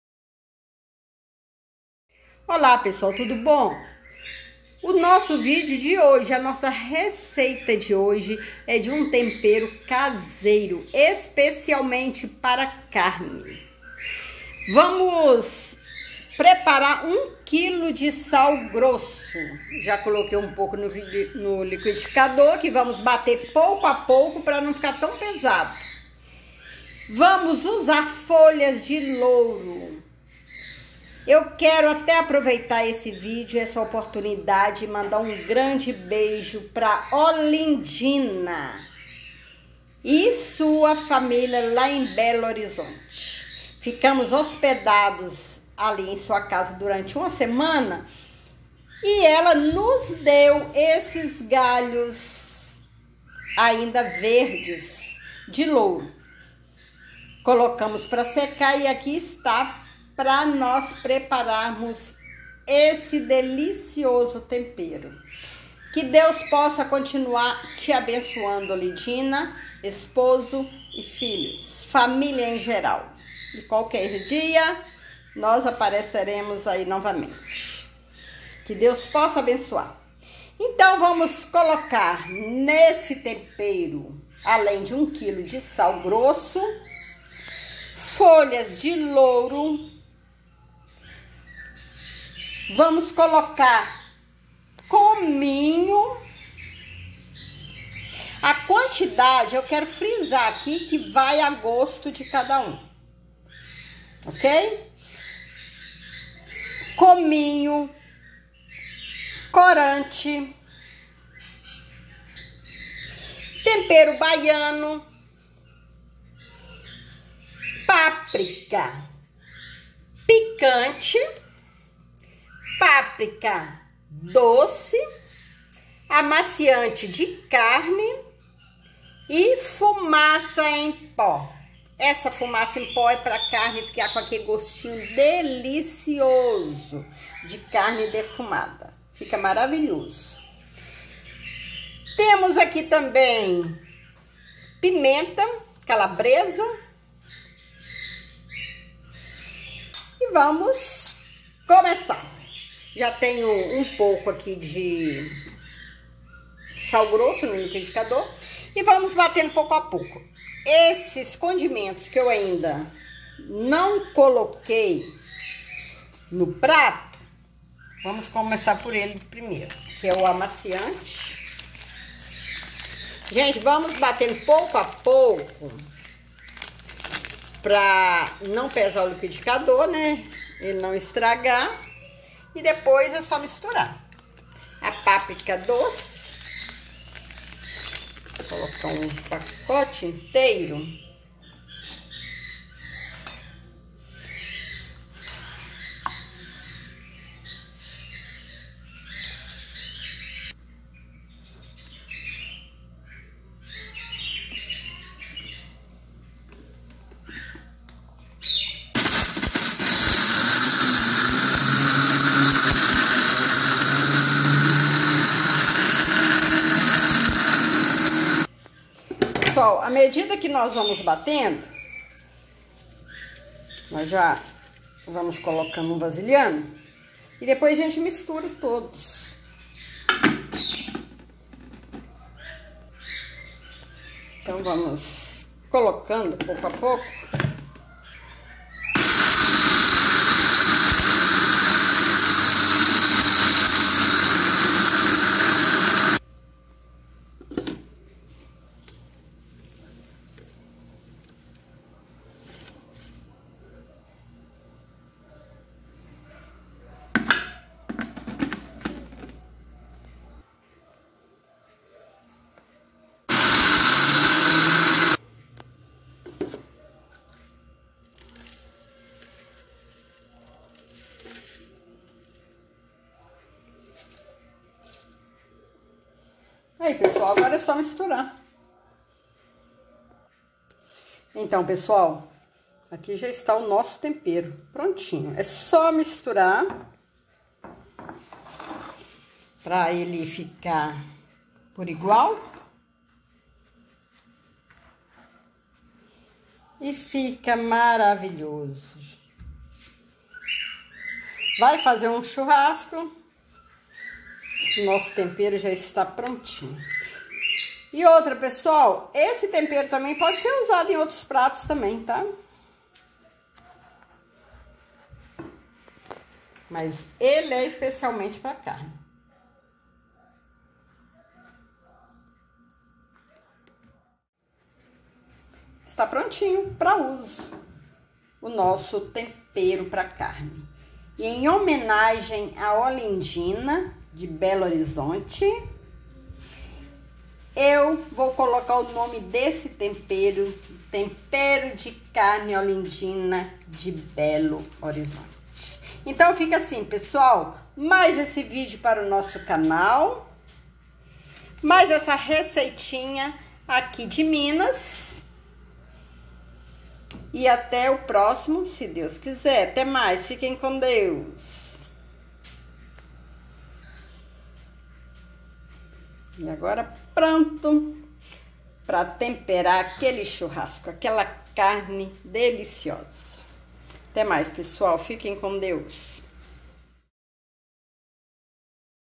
TEMPERO CASEIRO PRA CARNES - TIPO SAZON
TEMPERO CASEIRO PRA CARNES OLINDINA DE BELO HORIZONTE , usado em carnes de churrasco e também pode em outros pratos.  Excelente.

Veja na PLAYLISTS abaixo, mais de 150 dicas de RECEITAS PRÁTICAS E LANCHES

Experimente nossas receitas e nos diz se gostou.
Abraços